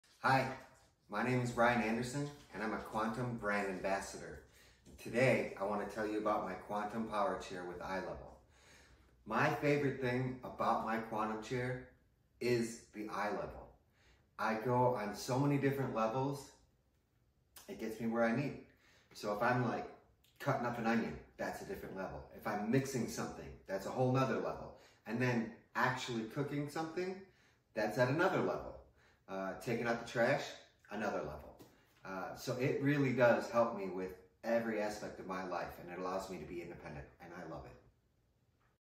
What I Love About My Quantum Chair - Bryan
Check out Bryan's video as he shares his favorite things about his Quantum Rehab chair! 🎥